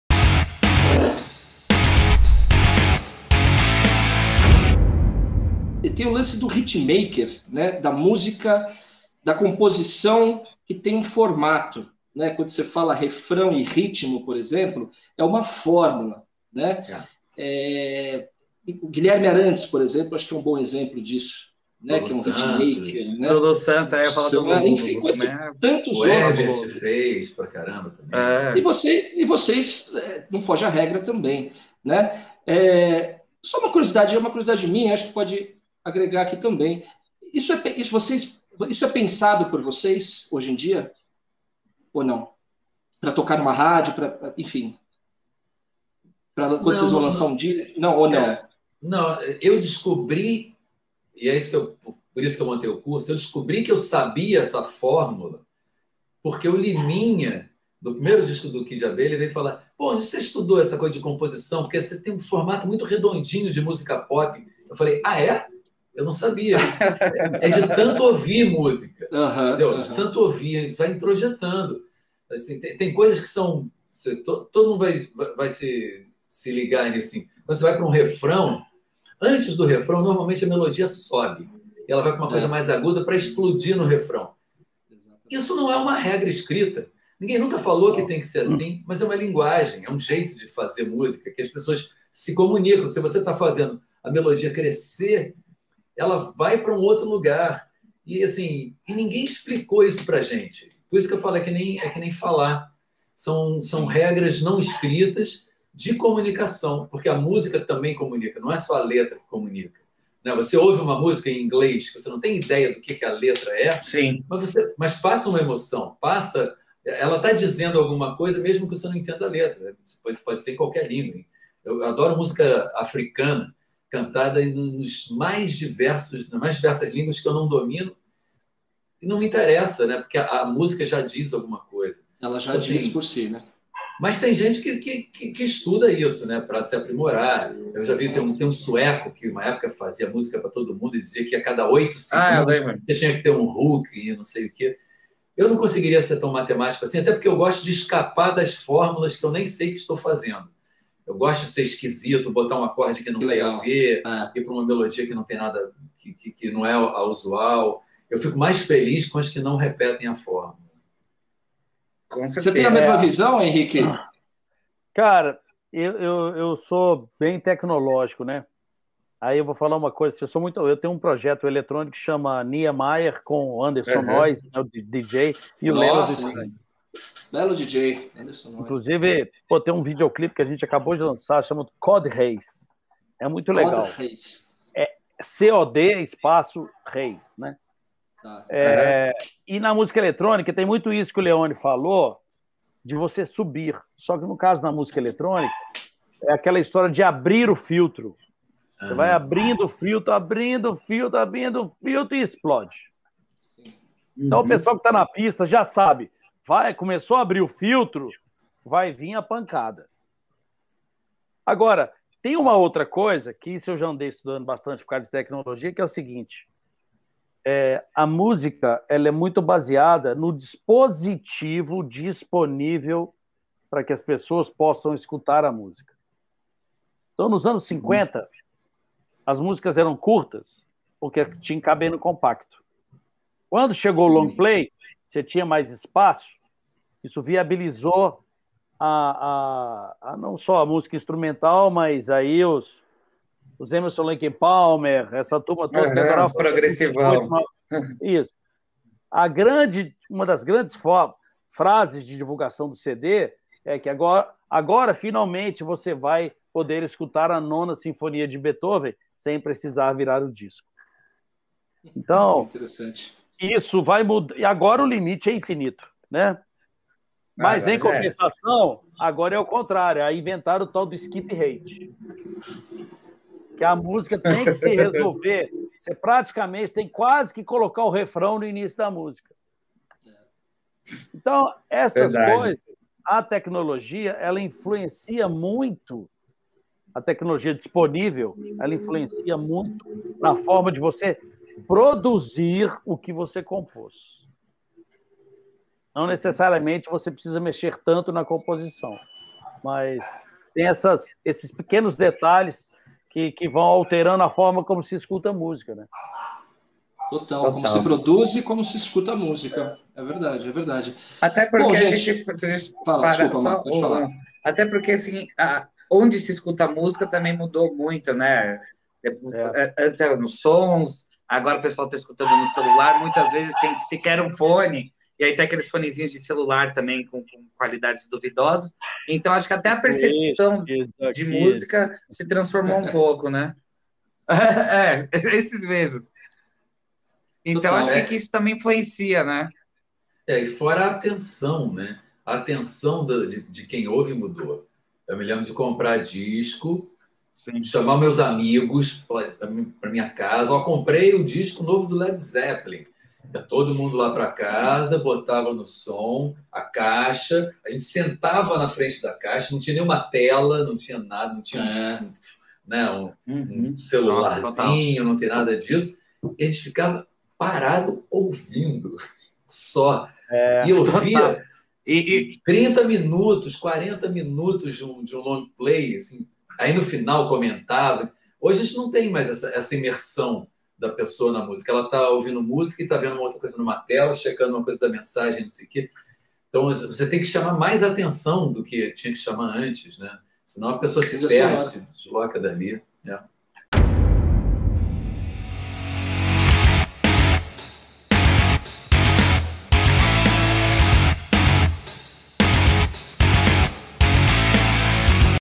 LEONI & HENRIQUE | A NECESSIDADE DE SE TORNAR UM HITMAKER
Quando questionado sobre as músicas do novo projeto serem hitmakers, Leoni conta que essa fórmula nunca foi ensinada a ele, mas que sempre esteve presente naturalmente em suas composições.

Já Henrique menciona que com as mudanças do público, muitas vezes os refrões são entregues no começo da música para conquistar o ouvinte e evitar o “skip rate”.